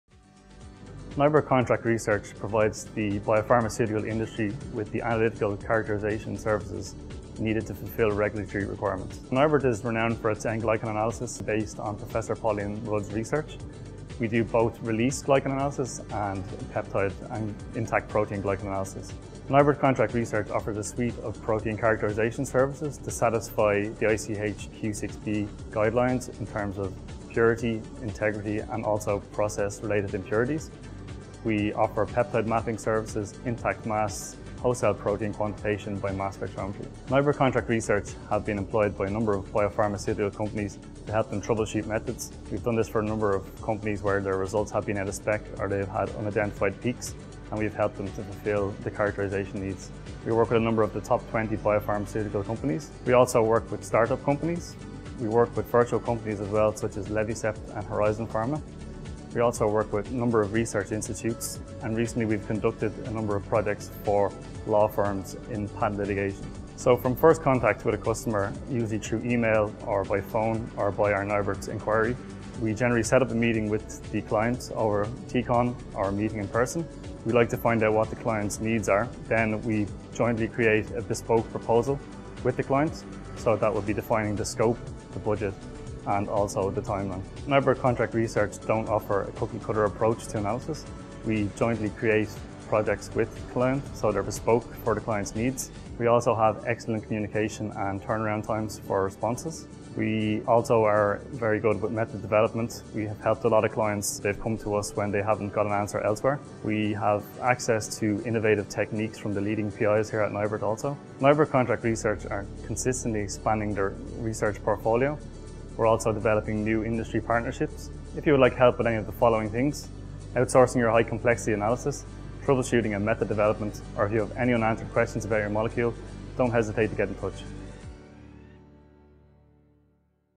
NIBRT Contract Bioanalytical Services
Our mission is to provide you with a partner to outsource analytical services. We provide invaluable information on your biologics, assisting you in key decision making during candidate development and process change.